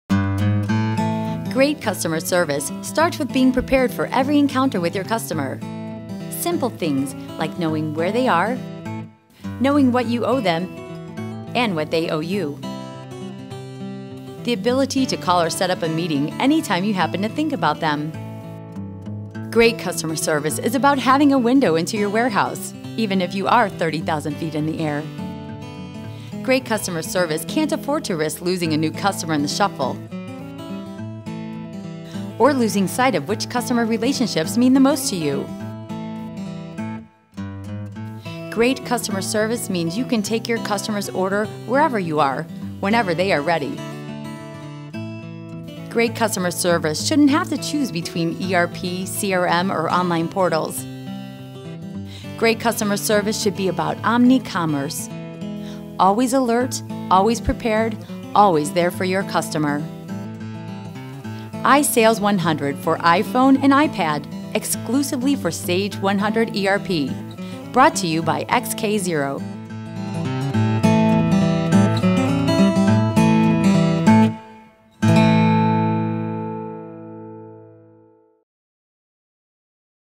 Sage 100 ERP on iPad and iPhone: iSales 100
Great customer service and increased sales today needs to accommodate the flexible lifestyles of you and your customer.  iSales 100, exclusively for Sage 100 ERP by xkzero.

All your Sage 100 (MAS 90/200) customers in your pocket.

iSales 100 =
  • Map
  • Appointment setting assistant
  • Email springboard
  • Phone book
  • Credit monitor
  • Recent activity
  • Customer Ledger
  • Living catalog
  • Your Price book
  • Order taker
  • Alerts
  • Window to your warehouse
  • Customer connection
  • More sales
  • Extraordinary customer experience
  • iPhone
  • iPad
  • iPod Touch
  • Apple
  • Situational CRM
  • Spanish language enabled